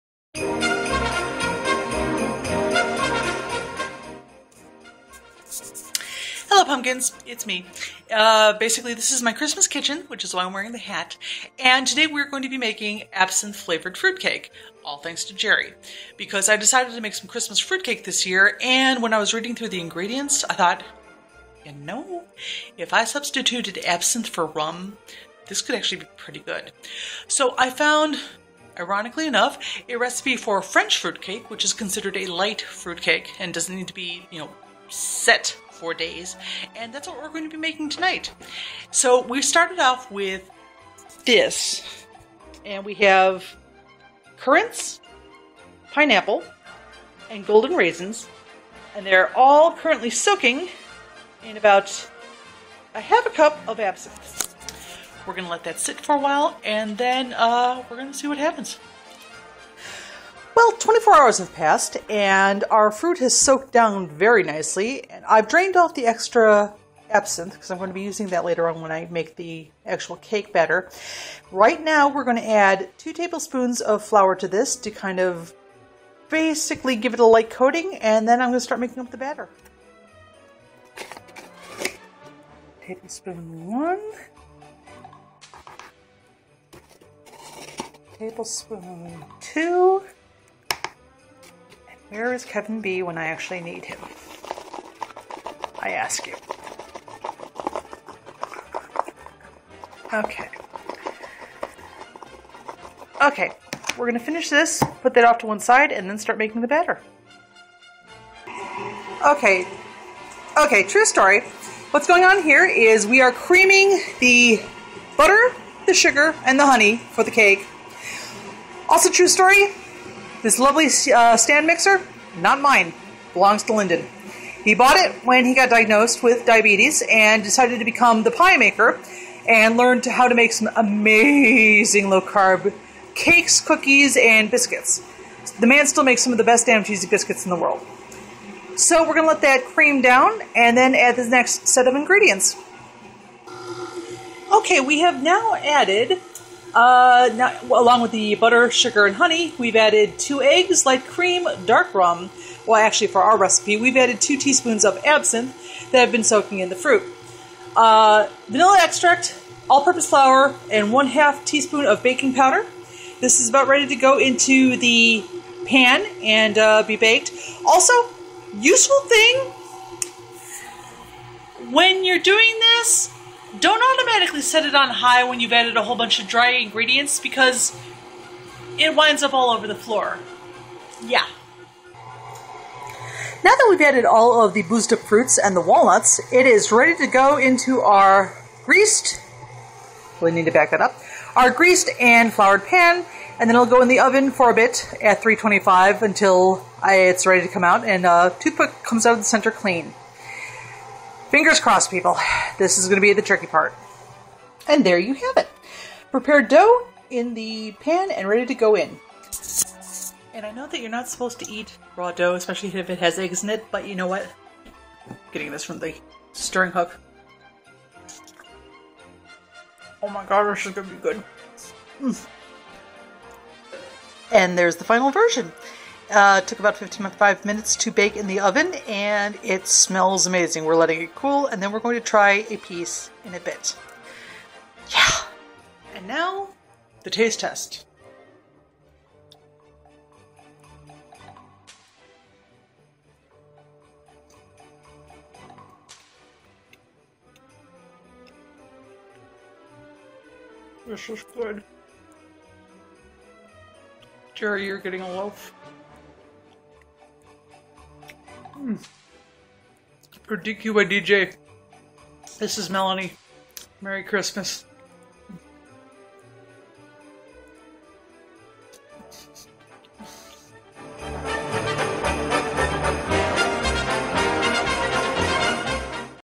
DQYDJ Videocast 11: Holiday Baking With Booze
Welcome to Videocast 11 of "Don't Quit Your Day Job: The Podcast!" where Melanie decides to take a crack at making a light French fruitcake using -- what else -- absinthe thanks to Jerry and his enabling ways. (And yes, strictly speaking it was recorded in 2012, but who's counting?)